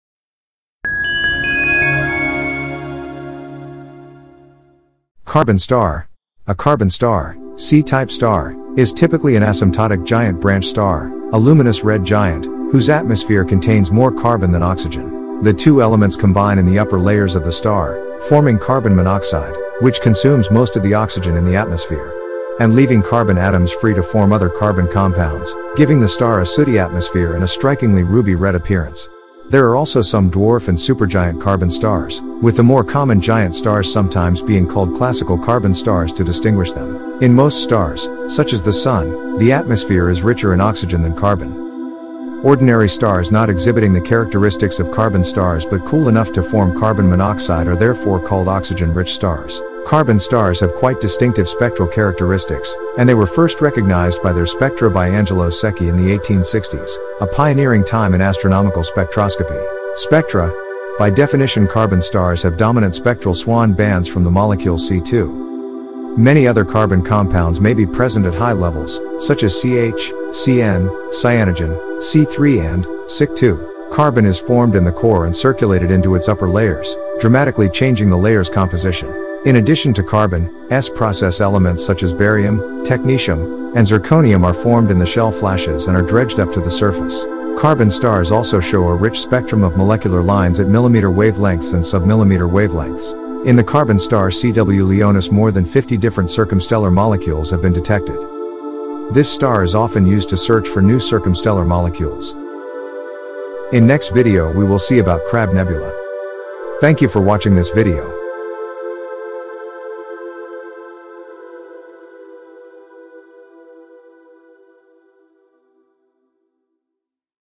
Carbon star
A carbon star (C-type star) is typically an asymptotic giant branch star, a luminous red giant, whose atmosphere contains more carbon than oxygen.
The two elements combine in the upper layers of the star, forming carbon monoxide, which consumes most of the oxygen in the atmosphere
And leaving carbon atoms free to form other carbon compounds, giving the star a "sooty" atmosphere and a strikingly ruby red appearance.
There are also some dwarf and supergiant carbon stars, with the more common giant stars sometimes being called classical carbon stars to distinguish them.
In most stars (such as the Sun), the atmosphere is richer in oxygen than carbon.
Ordinary stars not exhibiting the characteristics of carbon stars but cool enough to form carbon monoxide are therefore called oxygen-rich stars.
Carbon stars have quite distinctive spectral characteristics, and they were first recognized by their spectra by Angelo Secchi in the 1860s, a pioneering time in astronomical spectroscopy.
Spectra
By definition, carbon stars have dominant spectral Swan bands from the molecule C2.
Many other carbon compounds may be present at high levels, such as CH, CN (cyanogen), C3 and SiC2.
Carbon is formed in the core and circulated into its upper layers, dramatically changing the layers' composition.
 In addition to carbon, S-process elements such as barium, technetium, and zirconium are formed in the shell flashes and are "dredged up" to the surface.
Carbon stars also show a rich spectrum of molecular lines at millimeter wavelengths and submillimeter wavelengths.
In the carbon star CW Leonis more than 50 different circumstellar molecules have been detected.
This star is often used to search for new circumstellar molecules.
In next video we will see about  Crab Nebula.